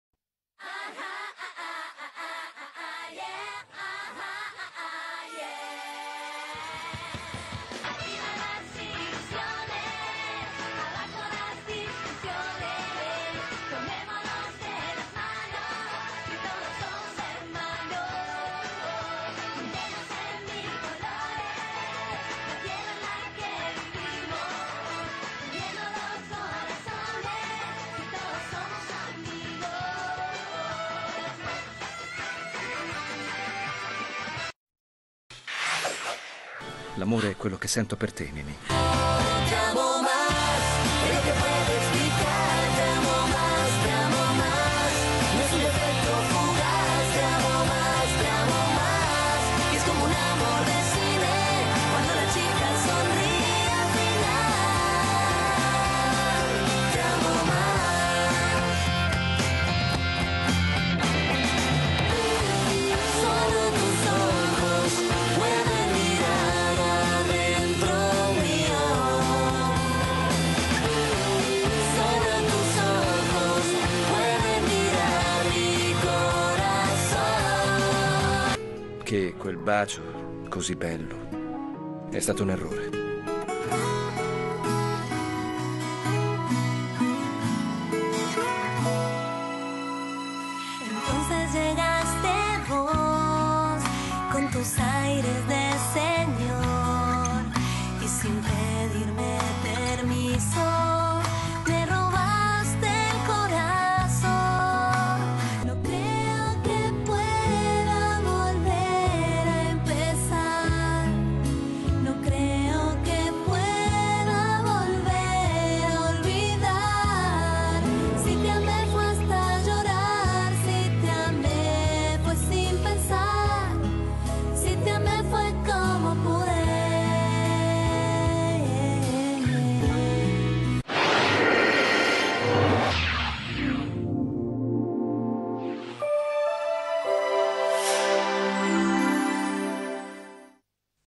Anteprima "Speciale San Valentino di Ninì" - Il 14 Febbraio 2012 su questo canale
Anteprima dello speciale di san Valentino di Ninì fatto interamente da me e che troverete il 14 febbraio 2012 per intero sul mio canale di YouTube "TheBoingTV". Non mancare a questo imperdibile appuntamento!